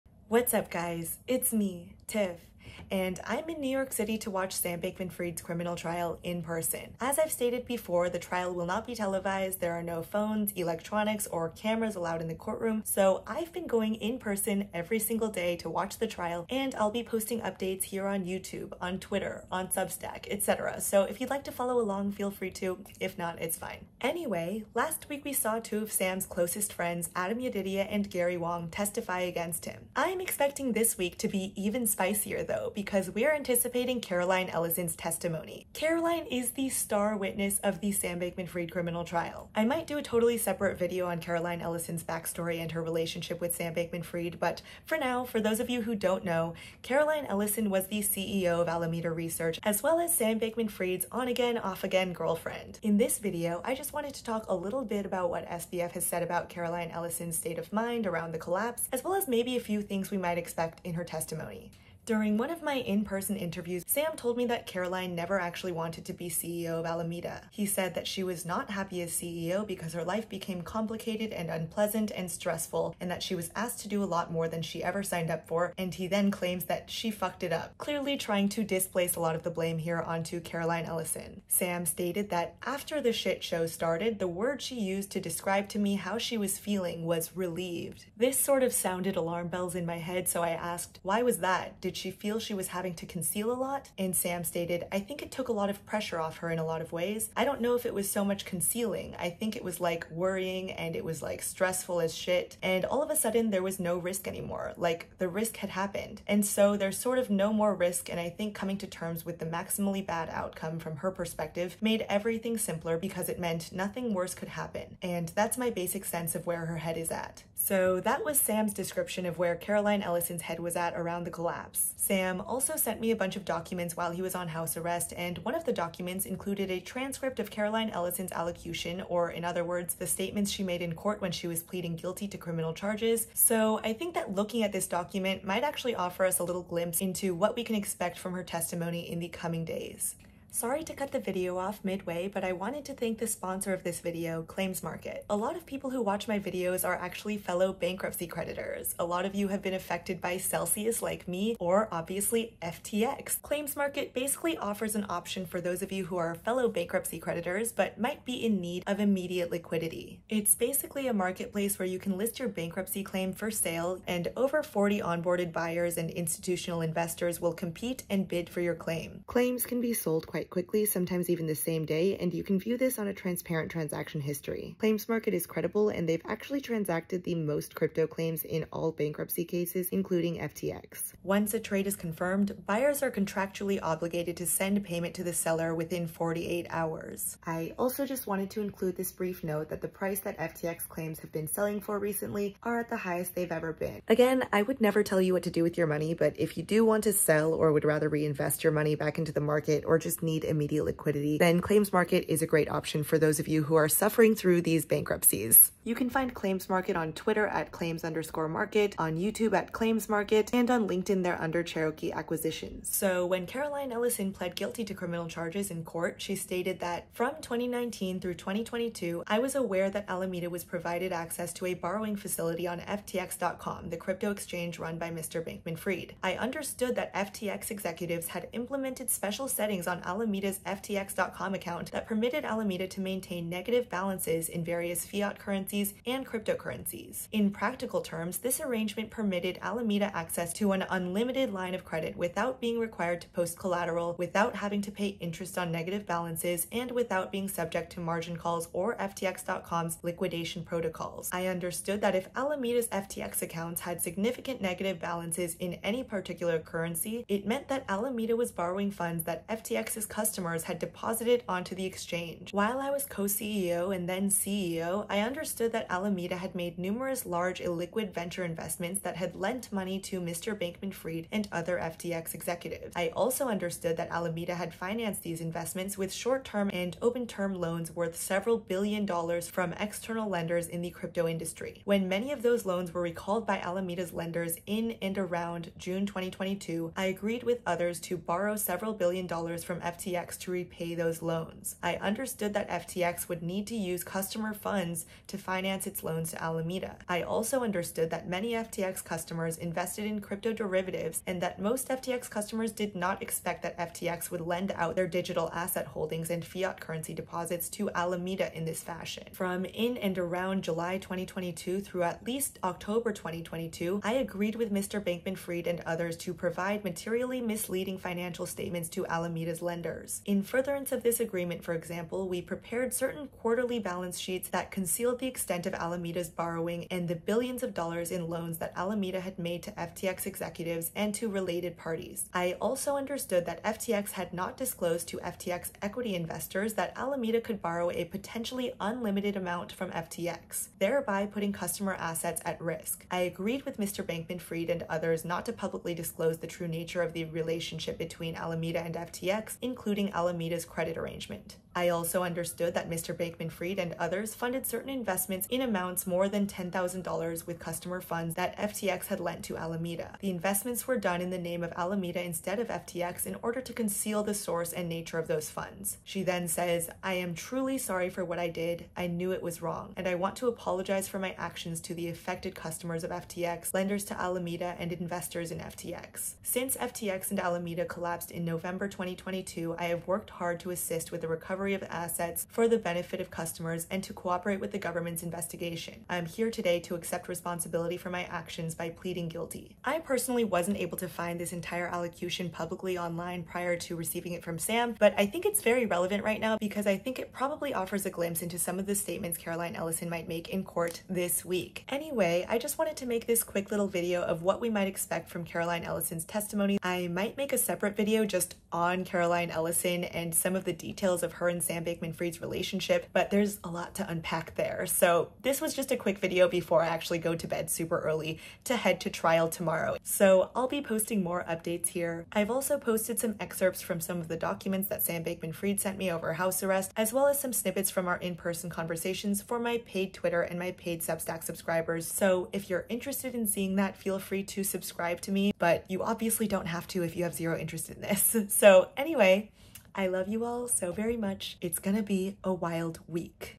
SBF Trial: Caroline Ellison's Testimony - What to Expect [LIVE FROM NYC]
I'm in New York City covering Sam Bankman-Fried's trial in person at SDNY courthouse. Caroline Ellison, SBF's ex girlfriend is expected to testify this week. Talking about some statements Sam made about Caroline after the collapse as well as discussing what to expect in her testimony.

⚡BTC: bc1qaprkyvtmvj0kdfy952t4dftwg755yqeajhlx5n
⚡ETH: tiffanyfong.eth

Please message me if you’ve donated so I can thank you personally!